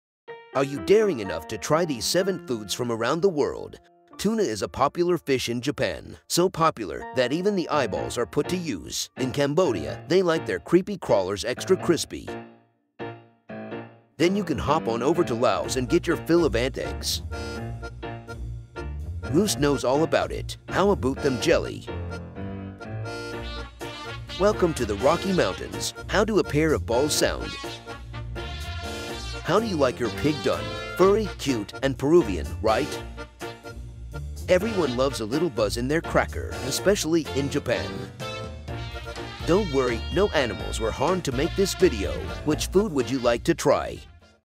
7 Weird Foods From Around The World
Are you ready to explore the weirdest foods from around the world? Let's take a culinary journey and discover 7 unusual dishes that you won't believe exist.